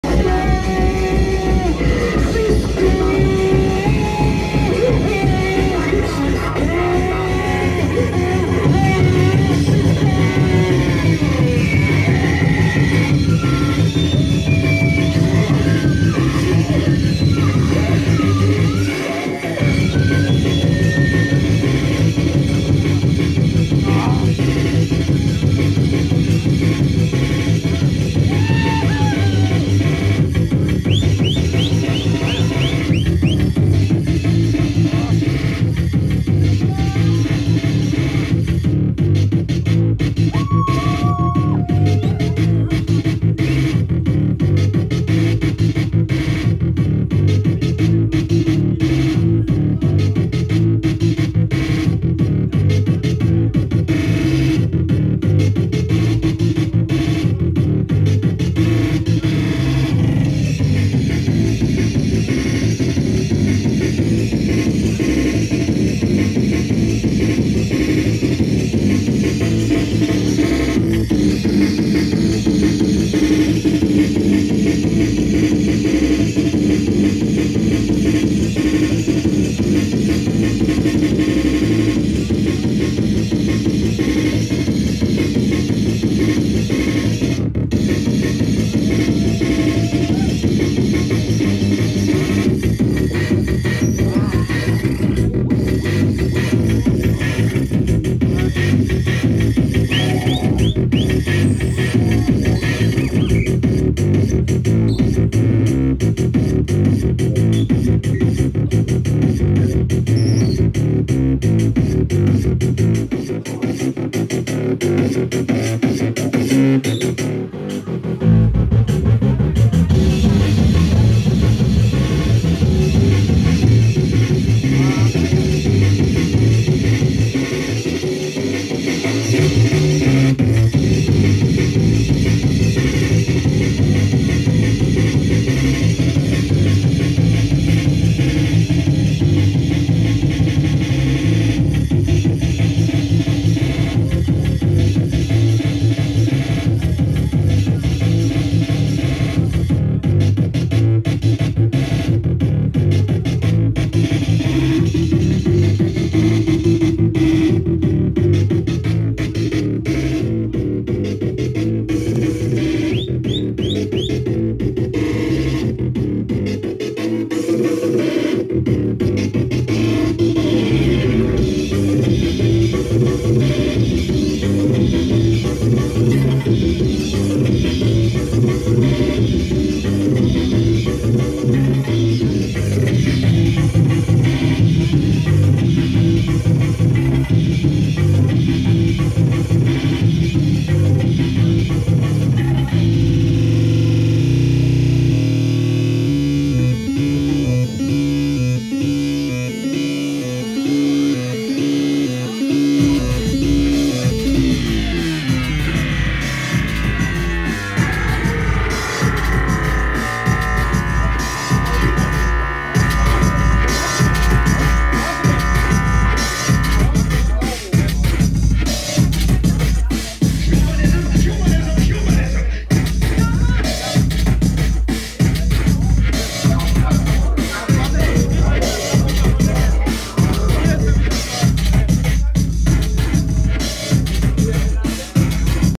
Der Totmacher @ scatty brainz Night 15.07.2022
Totmacher plays Breakcore as Shocking-Head and other Shit as Prof.Death @ this special Night  ;-)